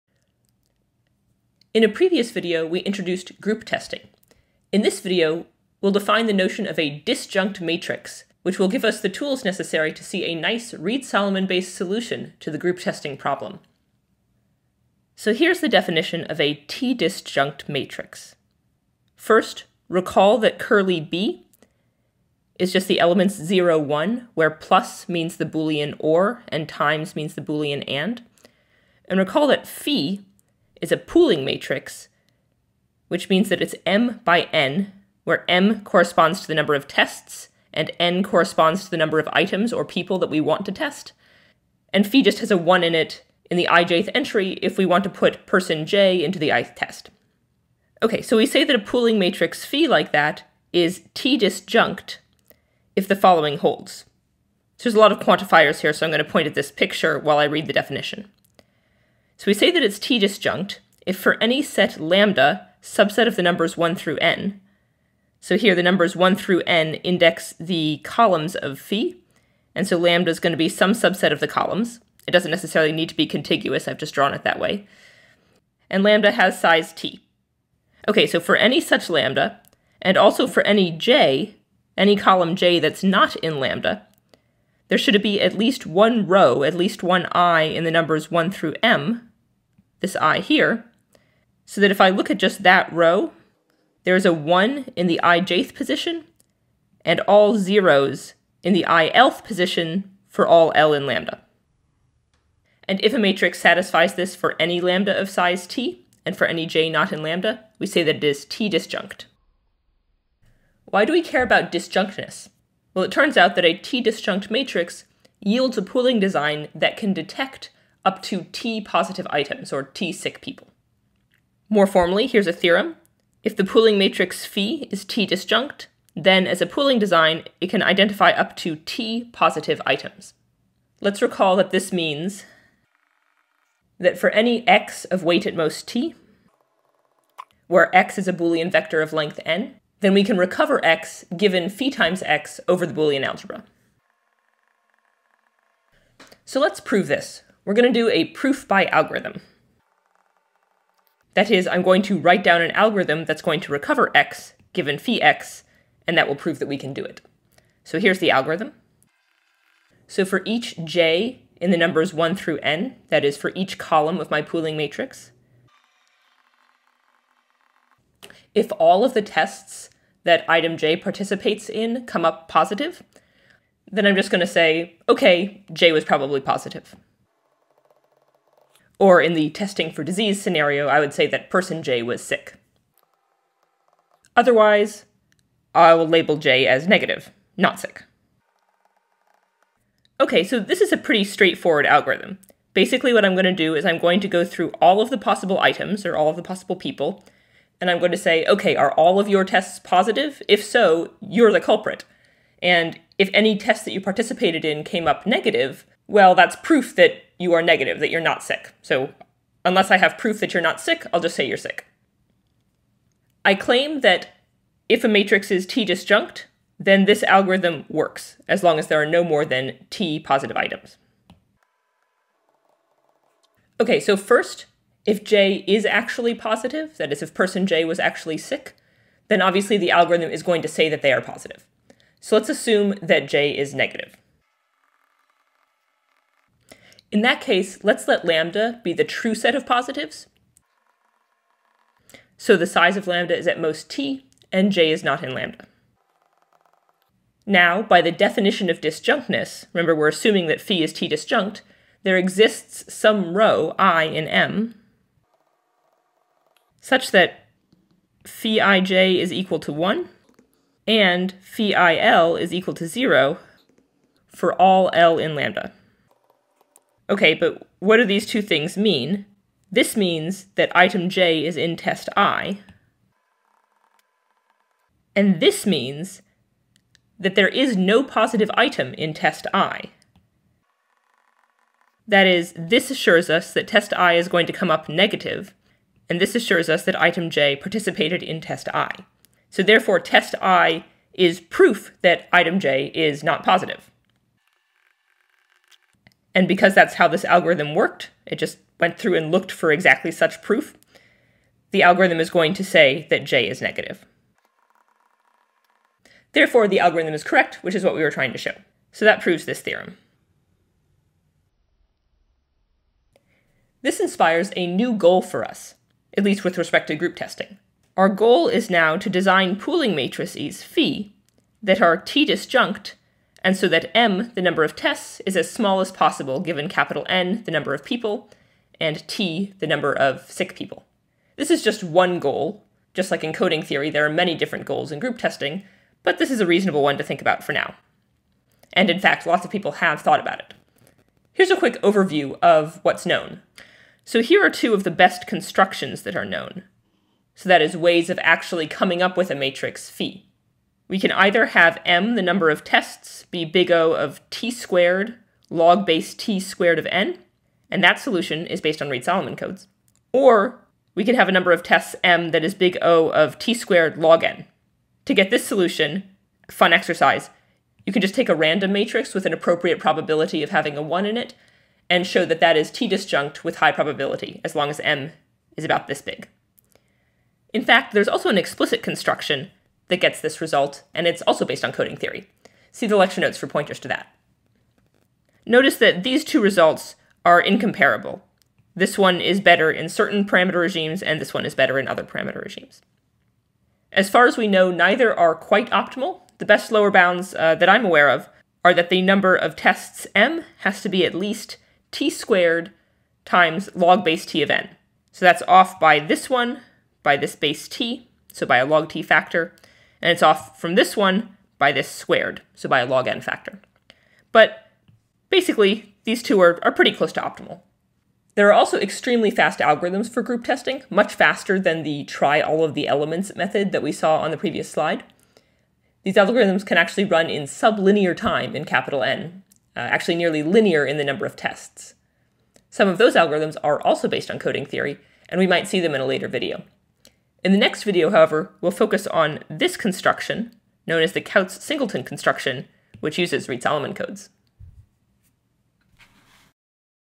Lecture 8, Video 2: Disjunct Matrices
We introduce a useful notion for thinking about the group testing problem.